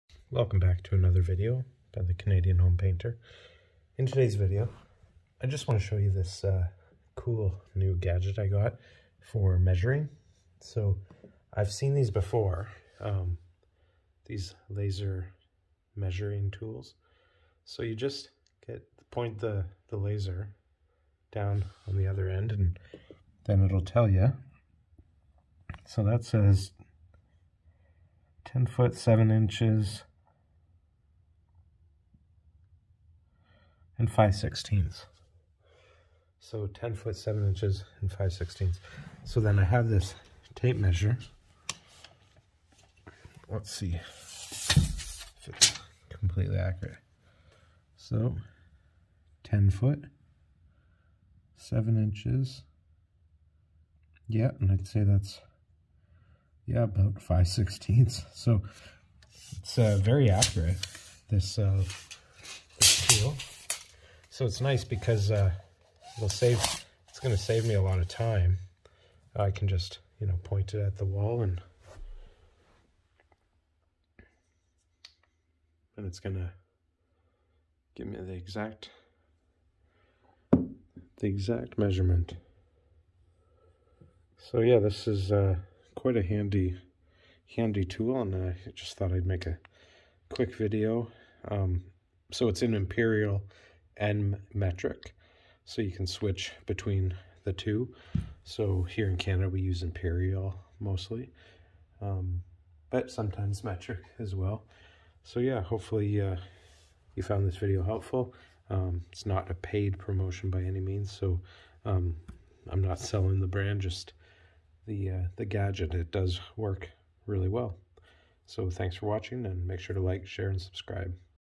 How accurate is a laser measure compared with tape measure
just a short video showing how accurate my new laser measure is, this was the first time I compared it to a tape measure